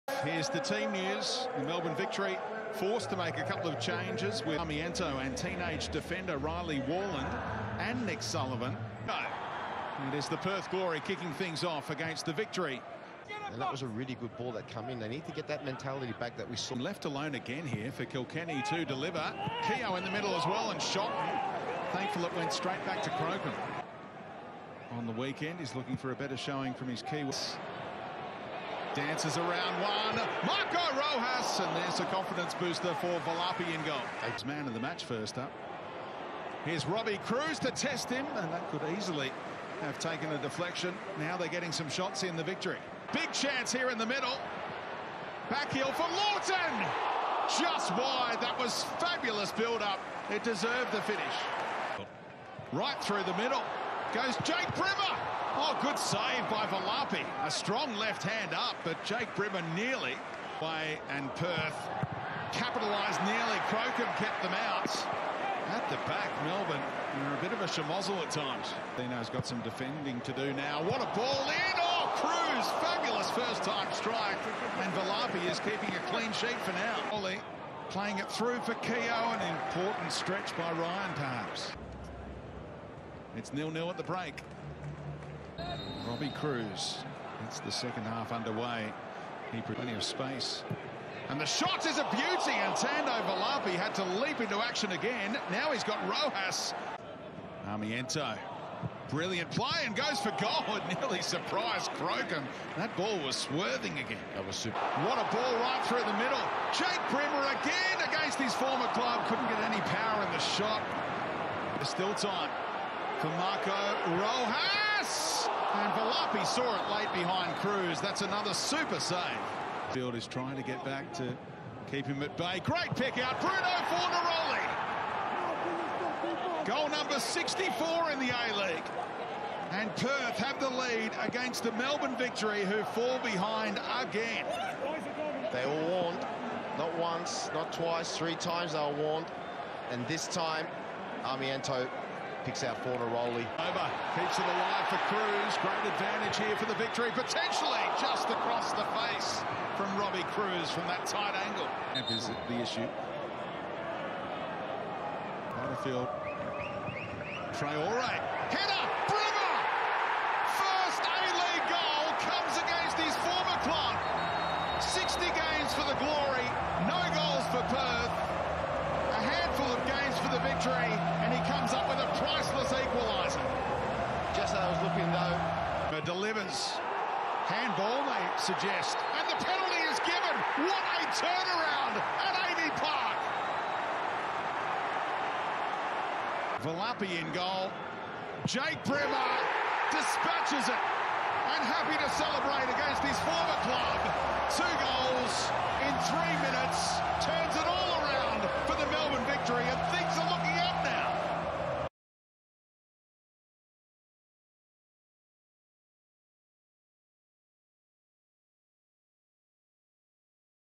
Melbourne Victory vs Perth Glory Highlights All Goals Hyundai A-League 26.01.2021
Melbourne Victory vs Perth Glory 2-1 Highlights All Goals Hyundai A-League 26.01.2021

Bruno Fornaroli 63'
Jake Brimmer 86'
Jake Brimmer 90' (pen)